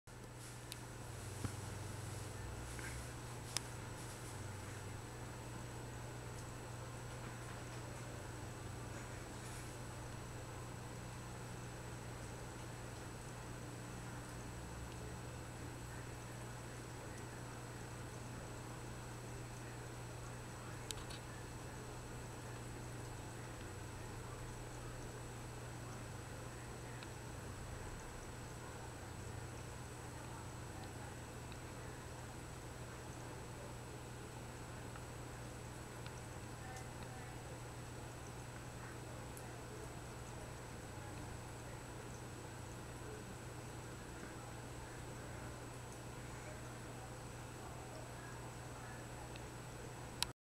Freshwater Limpet
A limpet snail nomming on algae. A freshwater limpet is a type of snail! It just have a clam-like top shell :O